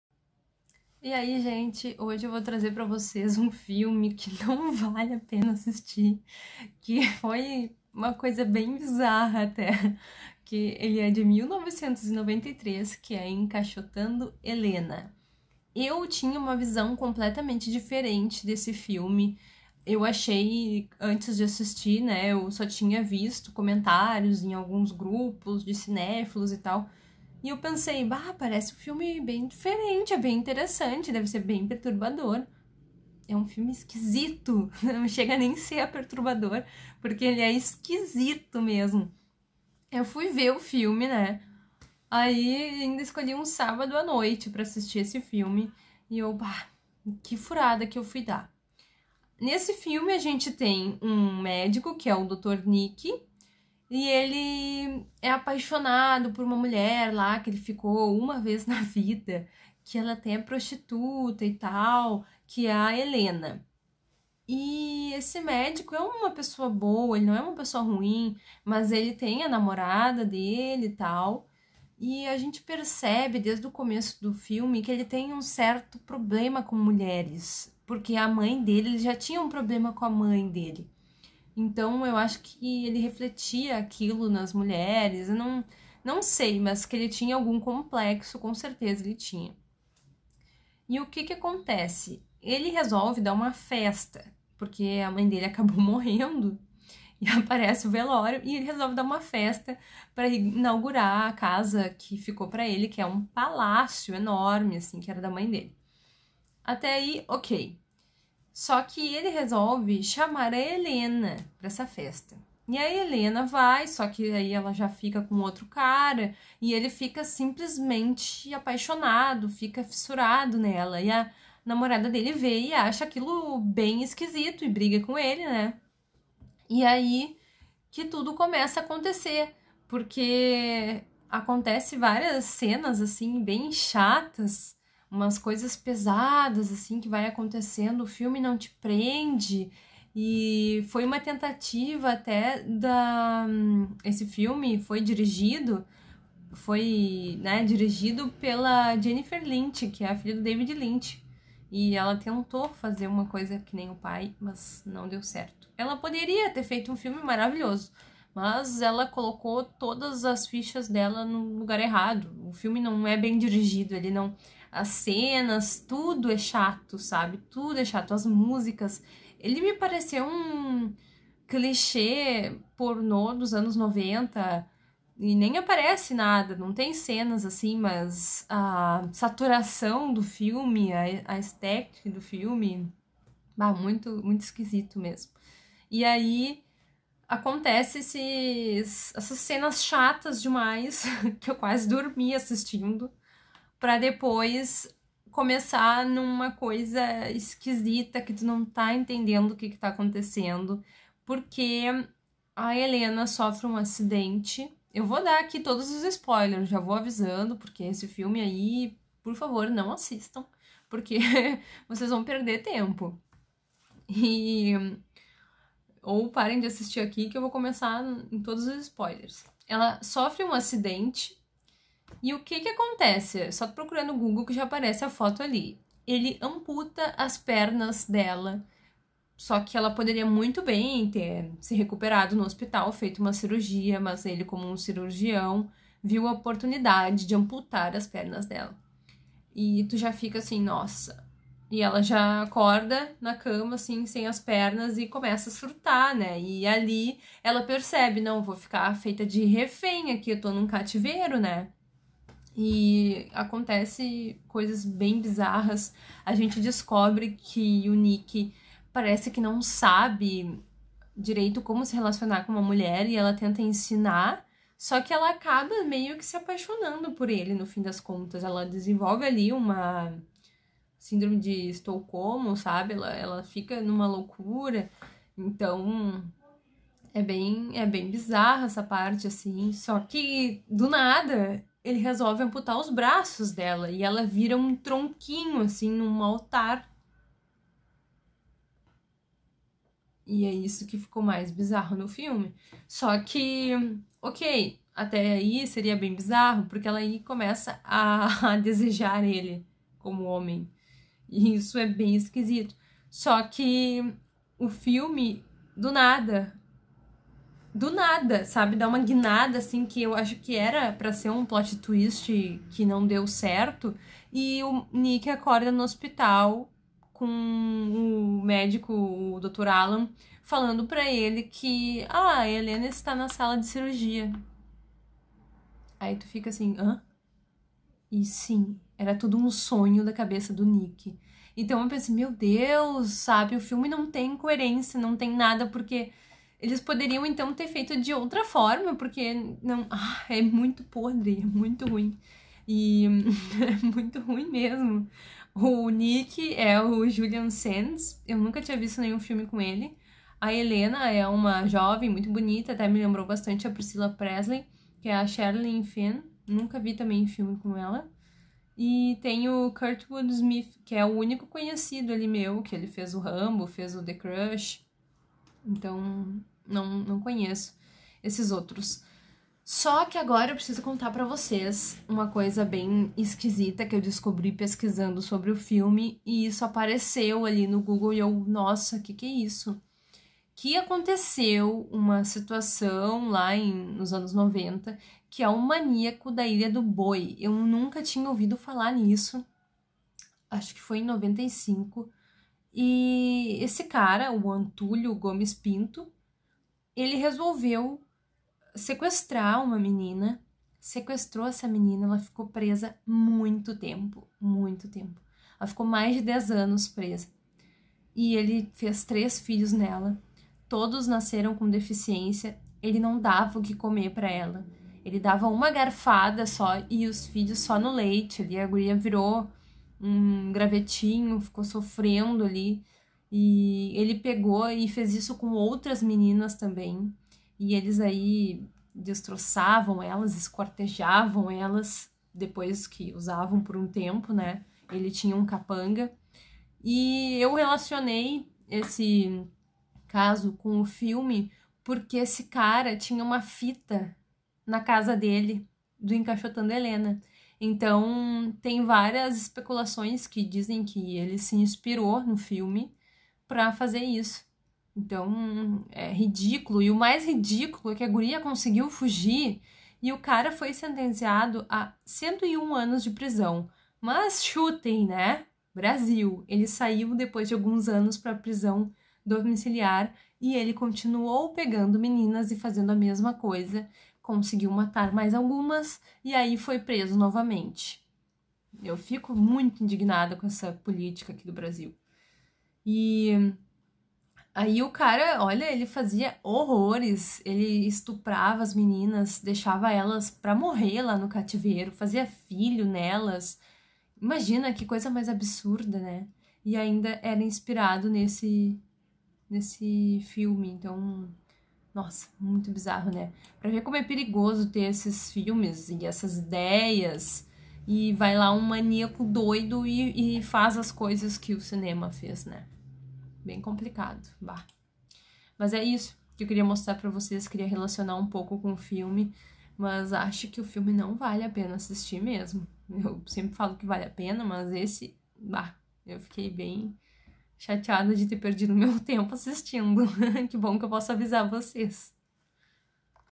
Encaixotando Helena 1993 e o Maníaco da Ilha do Boi
Filme bizarro que nos mostra os desejos mais profundos do Dr. Nick.
Filme com cortes ruins e péssima direção. Tem indícios que o maníaco da ilha do boi se inspirou no filme para fazer o que fez.